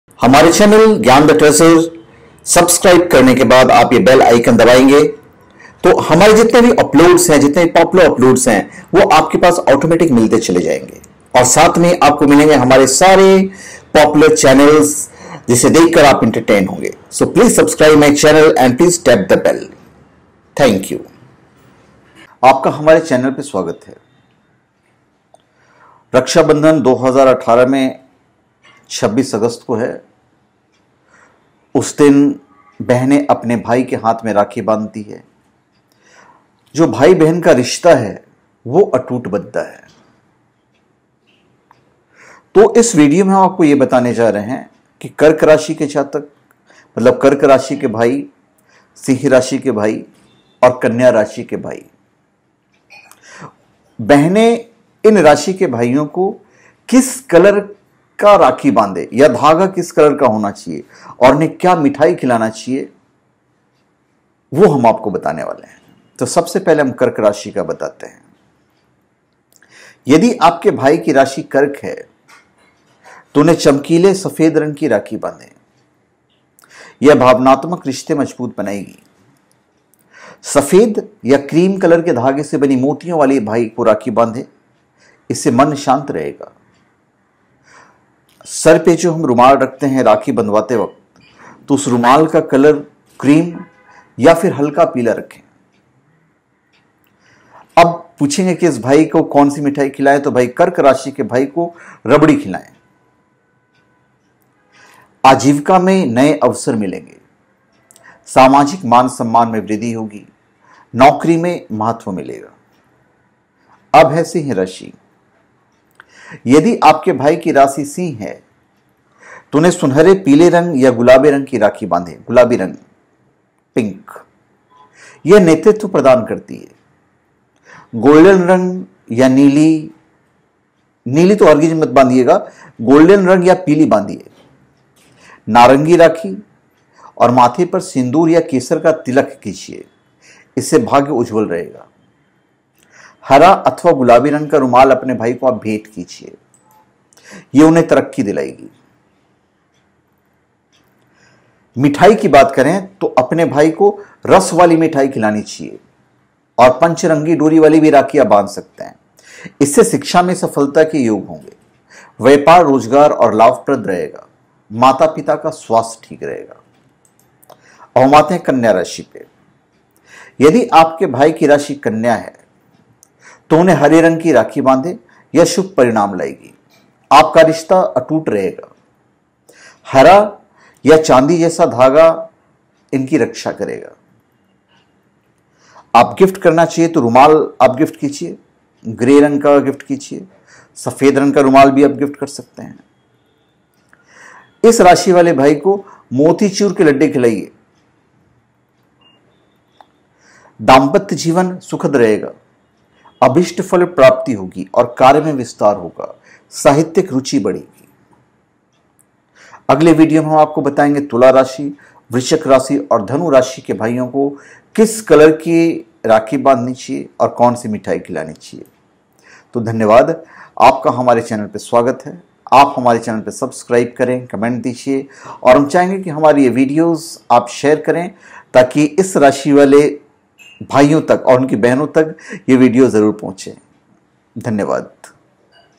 26 Aug रक्षा बंधन को कर्क, सिंह अाैर कन्या राशि के भाई को कौन सी मिठाई खिलाकर किस रंग की राखी बांधें
For Spiritual genuine authentic products visit my For Spiritual genuine authentic products Visit my 26 Aug रक्षा बंधन को कर्क सिंह अाैर कन्या राशि के भाई को क्या मिठाई खिलाकर किस रंग की राखी बांधें